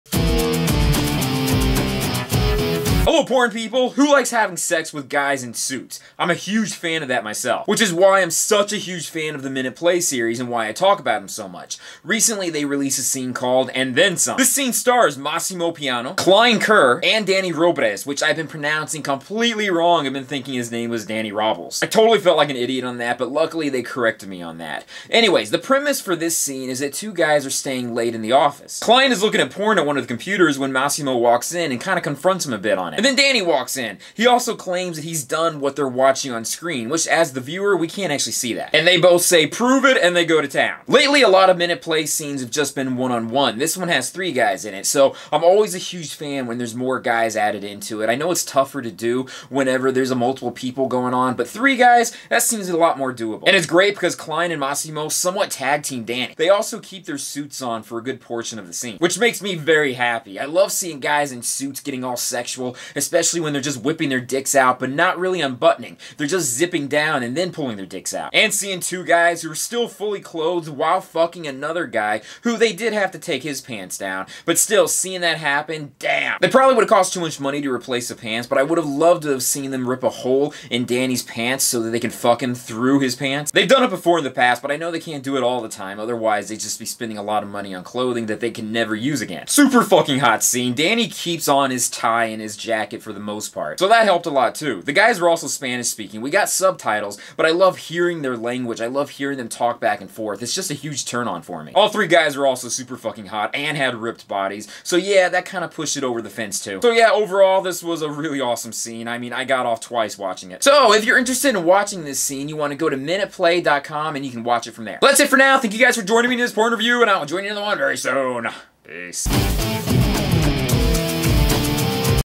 And Then Some - Men at Play Safe for Work Porn Scene Review
Who likes men in suits that fuck, while still wearing the suits? I do! That’s probably why I have such a big thing for Men at Play. This scene has 3 hottie guys that all keep most of their suits on for most of this scene.